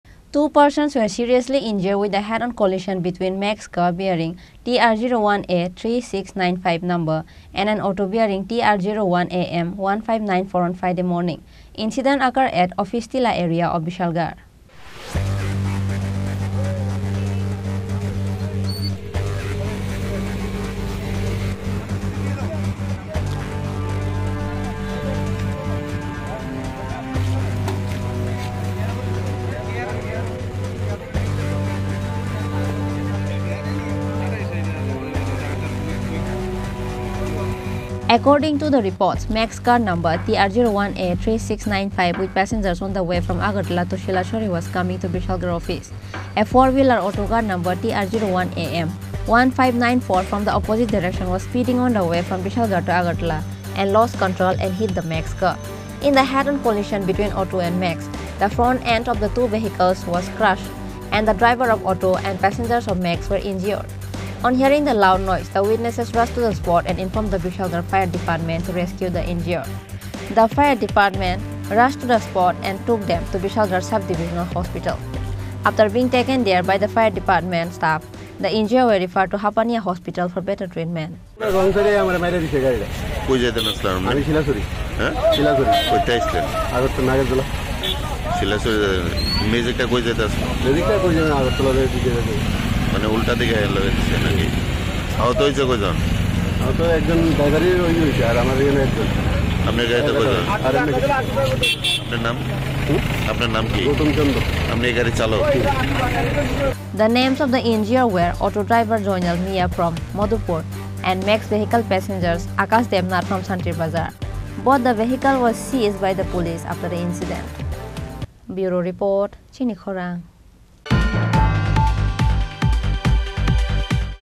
Two people seriously injured in a head on collision between a Max and an Auto on Friday
🟢 Two persons were seriously injured in a head-on collision between Max car TR01A3695 and an Auto  TR01AM1594 on Friday morning. Incident occurred at Officetila area of Bishalgarh.